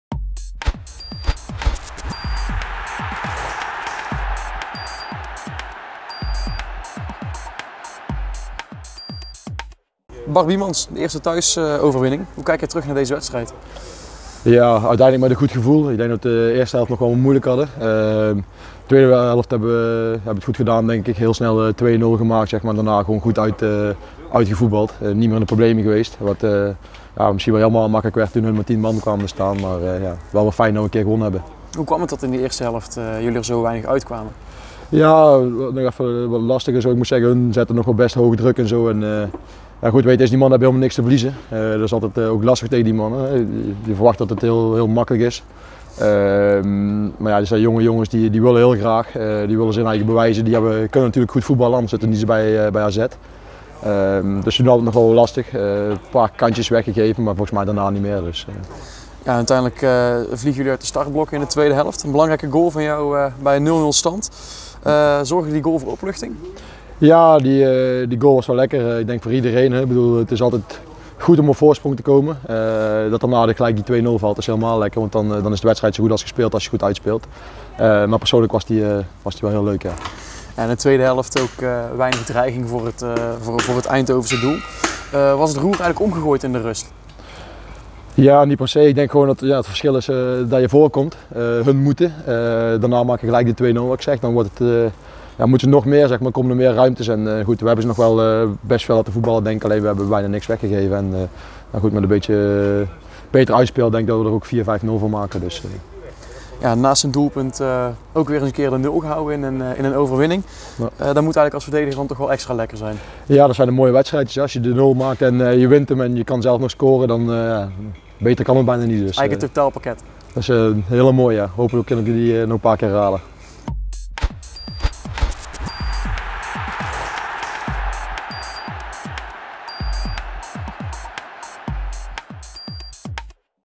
Bart Biemans: "Mooie wedstrijd. Gewonnen, de nul én gescoord." | INTERVIEW
Interview met Bart Biemans na de met 3-0 gewonnen thuiswedstrijd tegen Jong AZ.